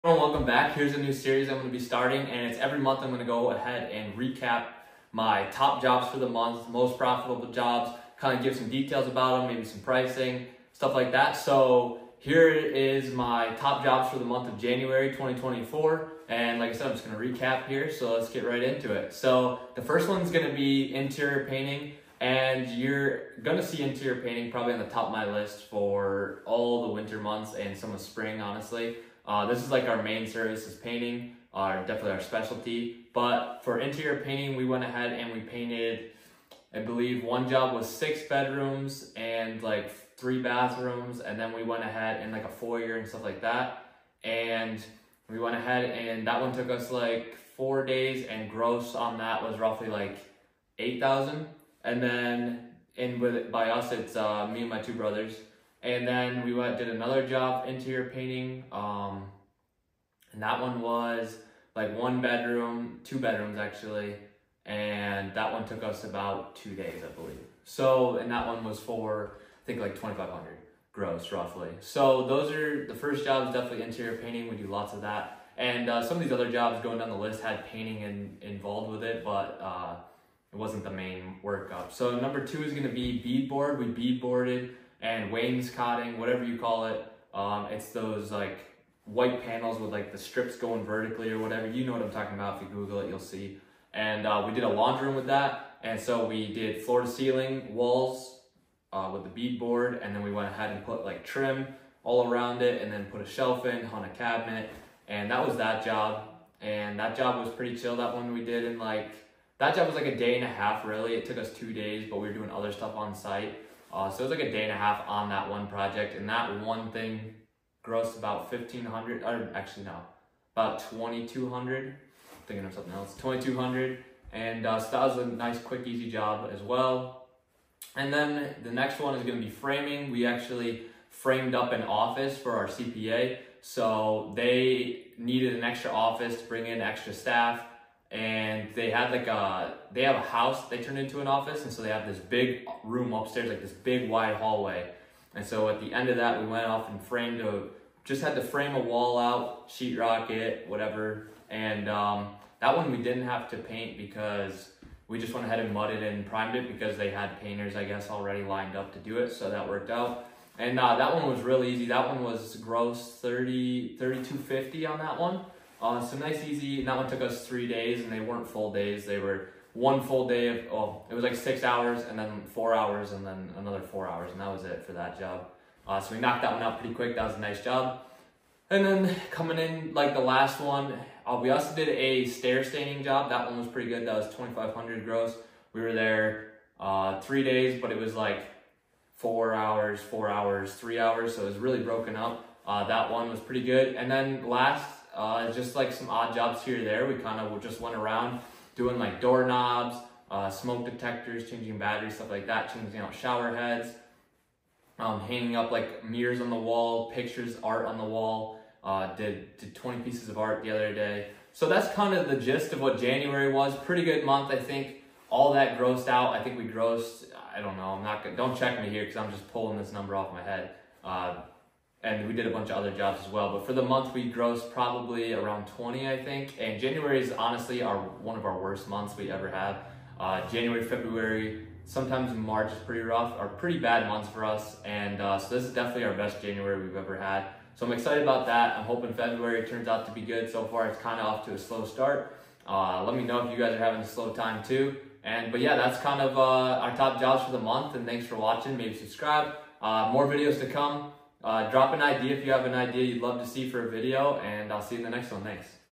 Business Recap| My Top Handyman Jobs For January 2024
Here are the top handyman jobs I did in January 2024. These winter months are always our slowest and we actually had a decent month this month.